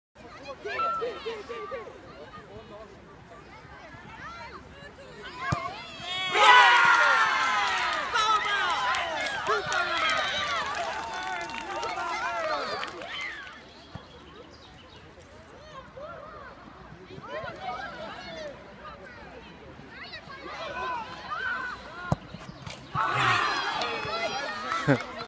Tyler's equalizer in the Duisburg South Cup
They were 1 goal down. They refused to give up. When Tyler pulled this solo attack, the way was open for the winner. In the end, they won 2-1. Great great game.